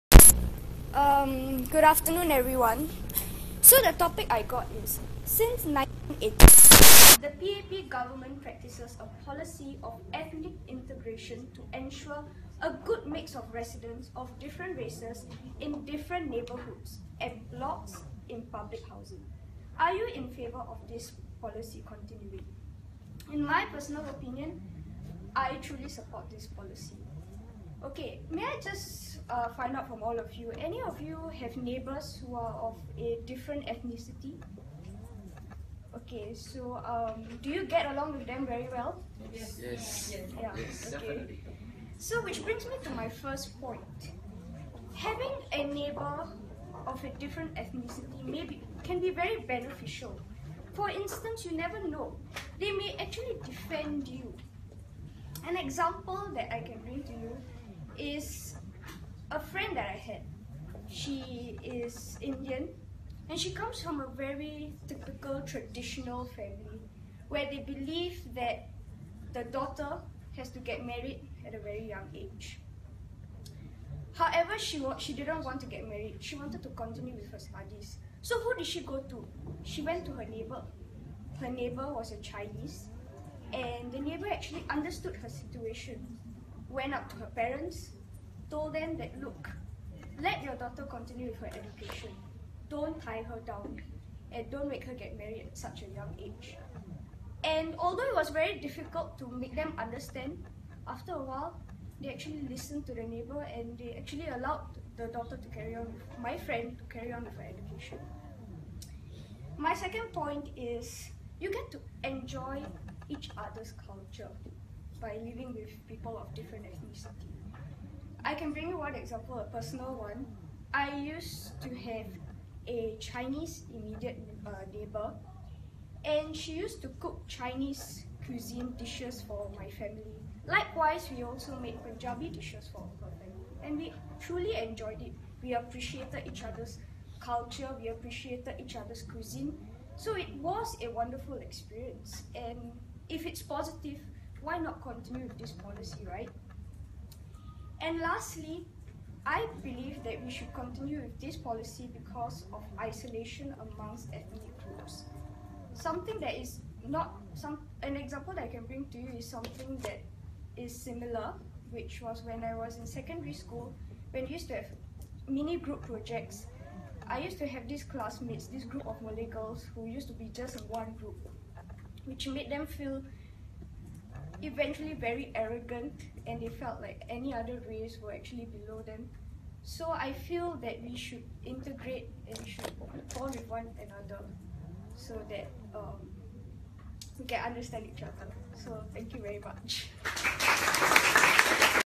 Example of an impromptu speech using the PREP framework
Topic: Since 1980, the PAP practices a policy of ethnic integration to ensure a good mix of residents of different races in different neighbourhoods and blocks in public housing. Are you in favour of this policy continuing?

Position: States definitively that she agrees with the statement.

Reason / Example: Uses 3 strong reasons + Examples to support her position.

Position: Restates her position.